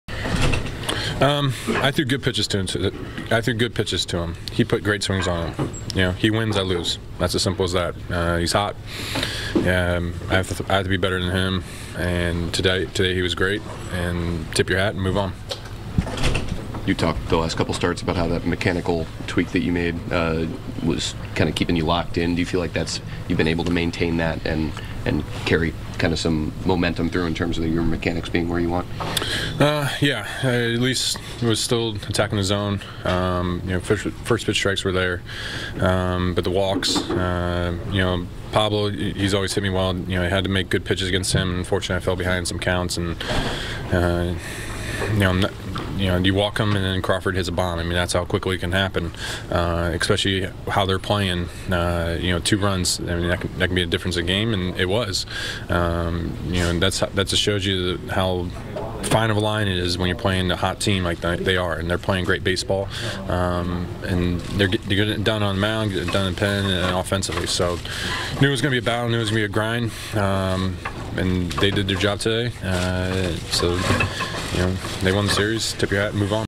Max Scherzer after his second loss of the season
Max Scherzer gave up just four hits through seven innings in his second loss of the season on Sunday. More